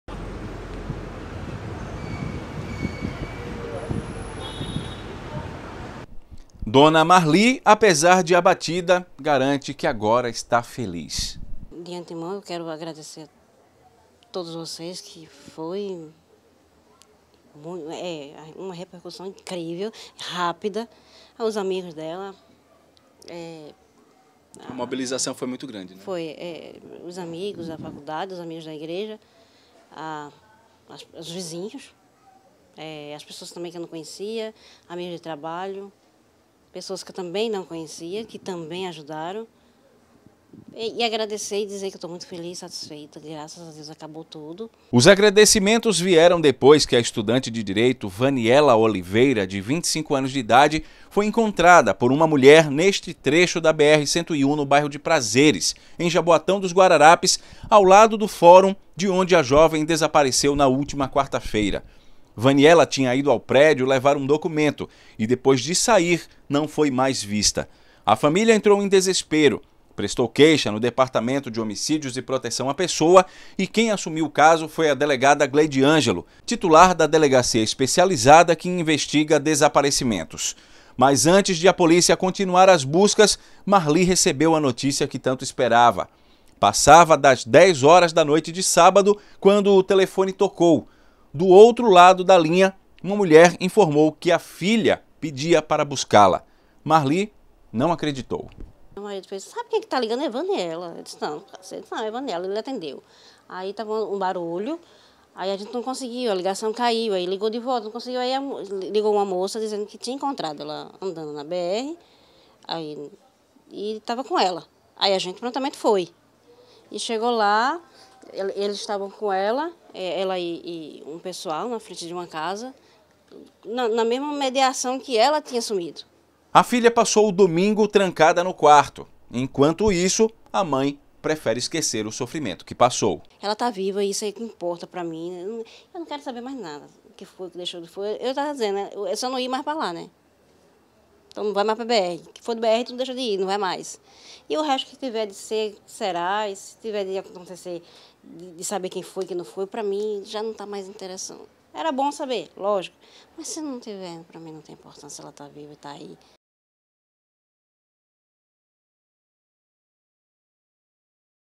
Mãe de Vaniela fala pela primeira vez após aparecimento da filha
Durante toda a entrevista, concedida na sala da casa da família, na UR-1, Ibura,  Vaniela permaneceu em seu quarto, segundo a mãe, dormindo. Marli disse que a filha estaria muito abalada e não teria conseguido parar de chorar para relatar o que passou nos últimos dias. A mãe contou que por volta das 22h recebeu o telefonema de uma mulher que teria encontrado a universitária chorando e teria se oferecido para ajudá-la, acionando a família. No reencontro, um forte abraço e lágrimas, mas nenhum relato. "Quando ela chegou em casa, a única coisa que disse foi: 'só quero tomar banho e dormir'", contou.

Imagens: TV Clube/Record